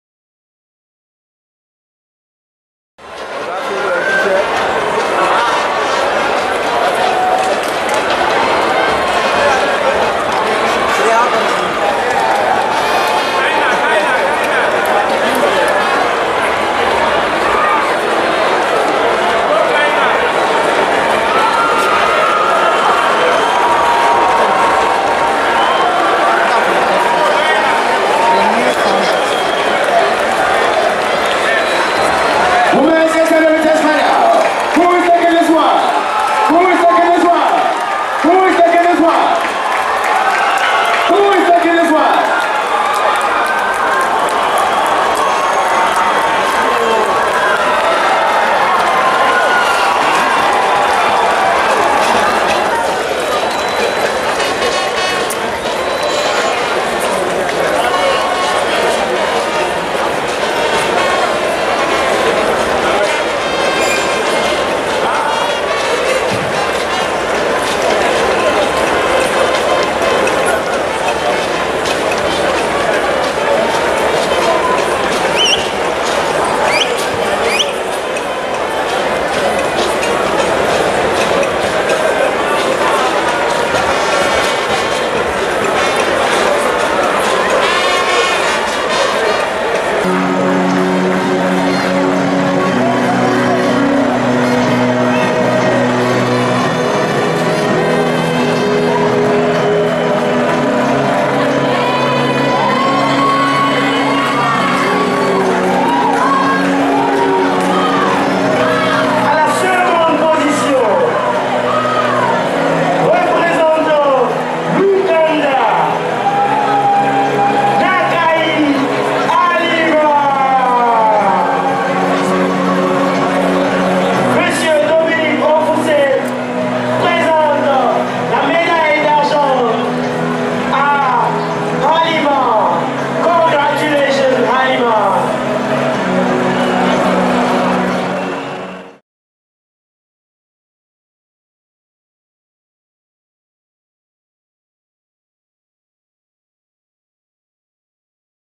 HALIMAH NAKAAYI wins Silver for Uganda in 800m - African Games Accra Ghana
Halima Nakaayi wins Silver for Uganda in 800m - African Games Accra Ghana